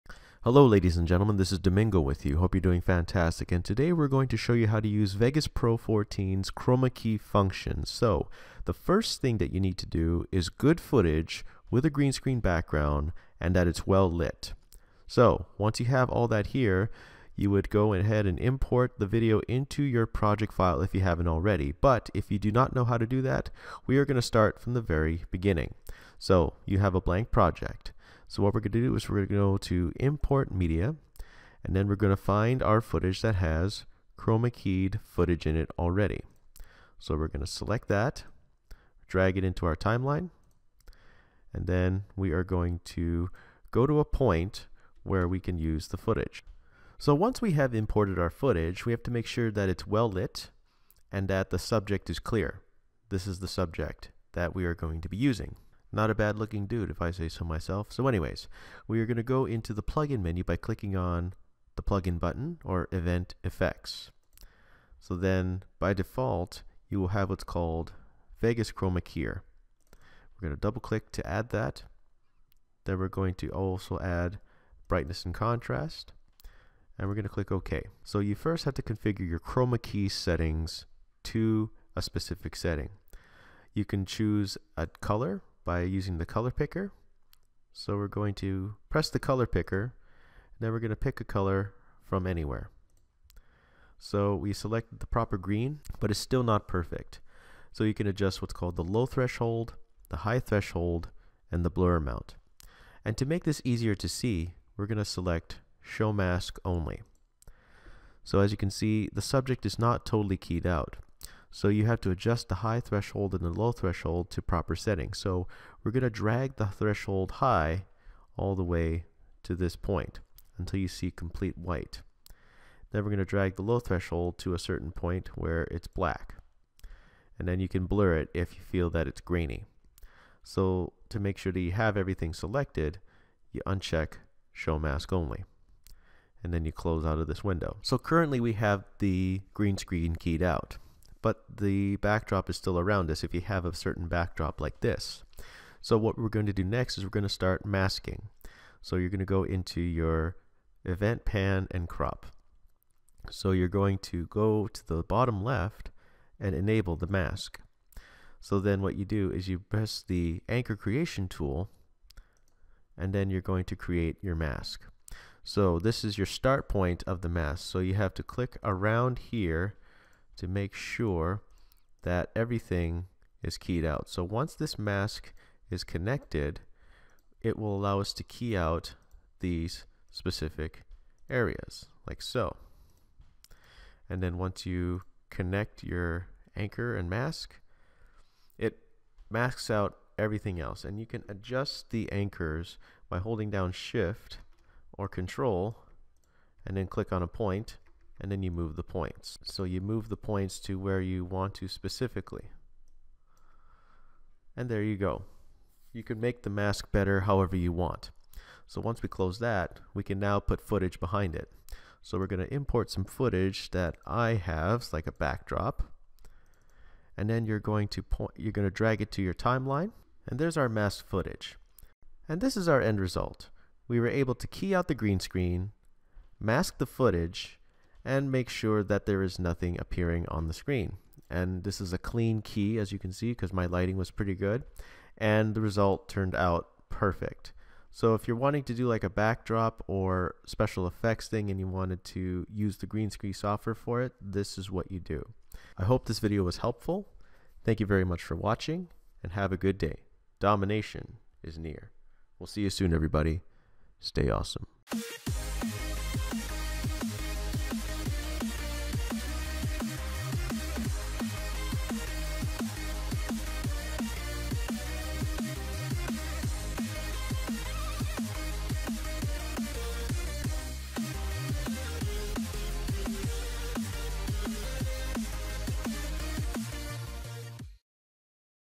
How to use Chroma Key in Vegas
Today we will teach how to use Chroma Key in Vegas Pro 14

Did you see this in your sub box?